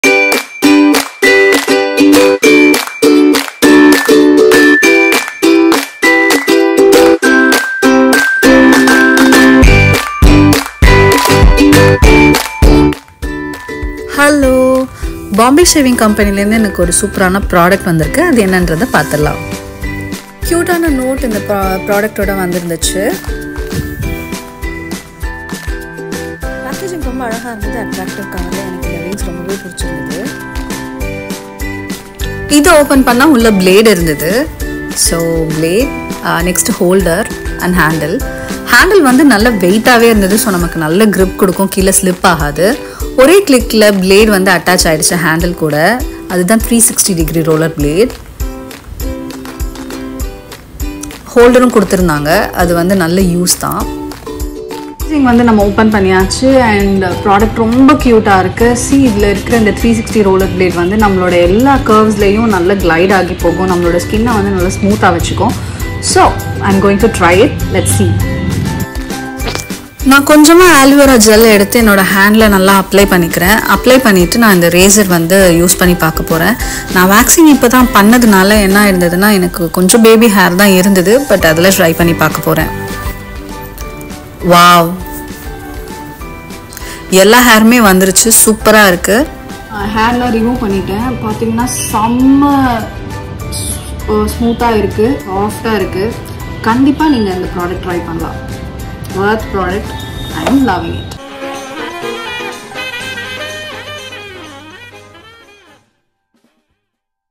Get smooth with Bombae rollplay razor | Testing the rollplay razor | Review
Hello everyone

Get ready to roll with the Bombae Roleplay Razor by Bombay Shaving Company! 🌟 Experience the ultimate smoothness with its 360-degree roller blade that effortlessly glides through curves, leaving your skin moisturized and radiant. Say hello to your new shaving companion! 💫

Musician: Marphologiya